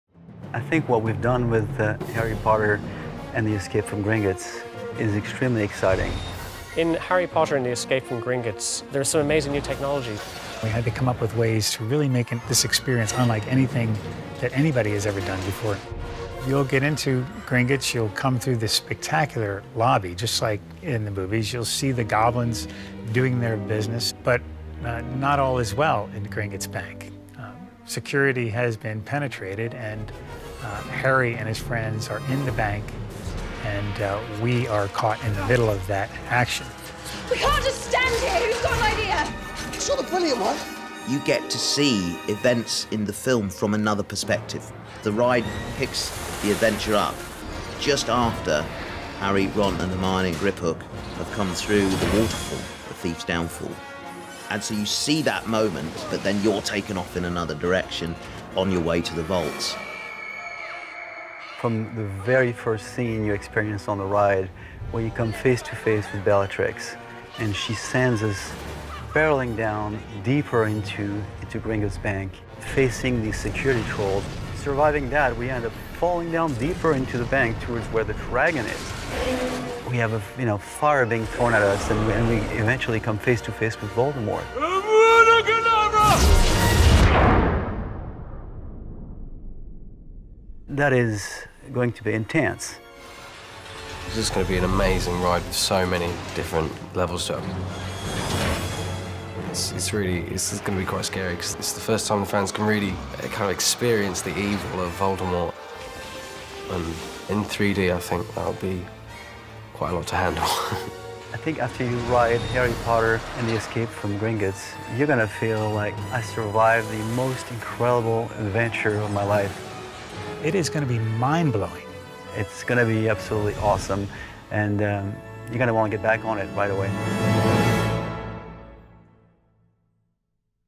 Harry Potter and the Escape from Gringotts - Behind the Scenes | Universal Orlando
Harry Potter and the Escape from Gringotts will be the signature ride of The Wizarding World of Harry Potter -- Diagon Alley. It will be a groundbreaking, multi-dimensional thrill ride that will place riders in the middle of an incredible adventure as they navigate their way through the Gringotts vaults.